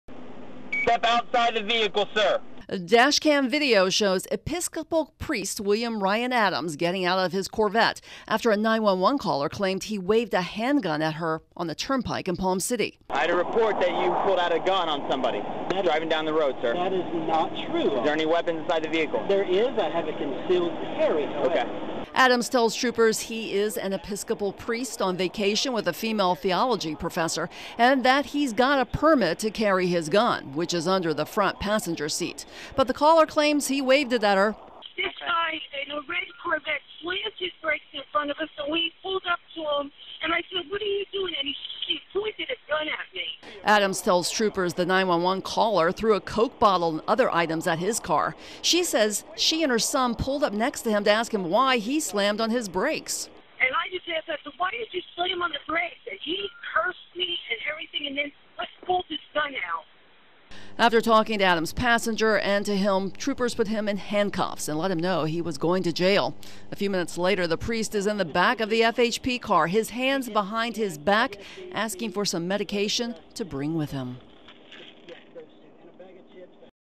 Dashcam Video of Gun Wielding Priest's Arrest
in Florida